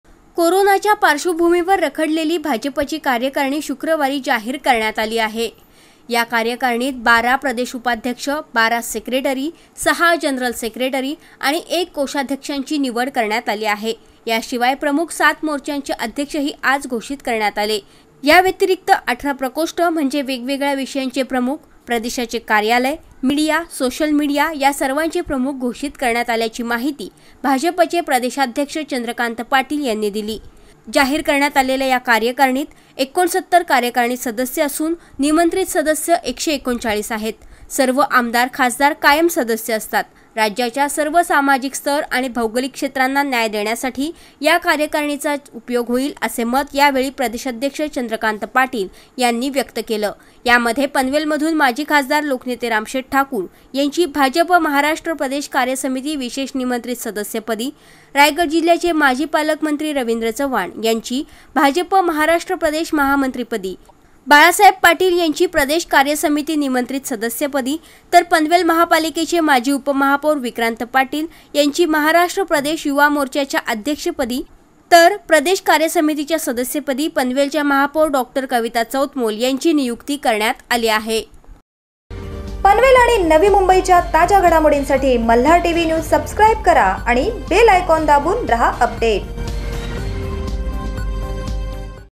malhartvnews | भाजपची प्रदेश कार्यकारणी जाहीर !
malhartvnews at panvel | भाजपची प्रदेश कार्यकारणी जाहीर;
माजी खासदार लोकनेते रामशेठ ठाकूर यांची महाराष्ट्र प्रदेश कार्यसमितीच्या विशेष निमंत्रत सदस्य पदी तर विक्रांत पाटील यांची प्रदेश युवा मोर्चाच्या अध्यक्ष पदी नियुक्ती !